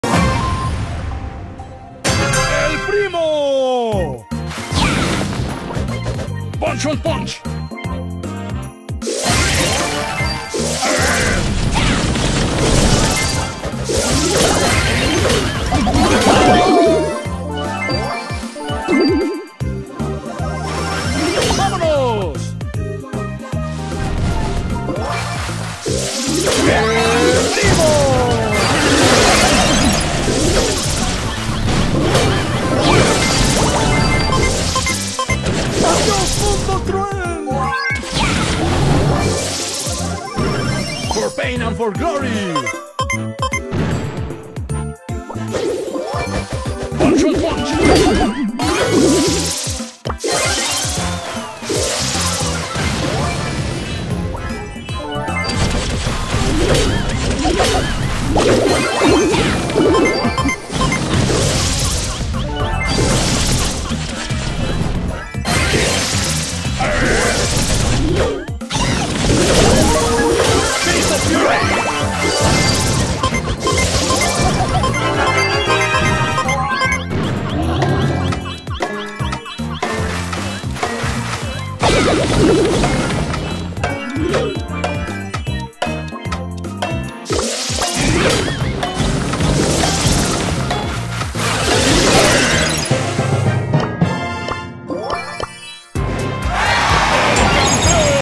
Insane fighting Brawler meta win!!!
Me, Bekfast (on another account), and Haydenash23 pick fighting Brawlers and go ham on the other team.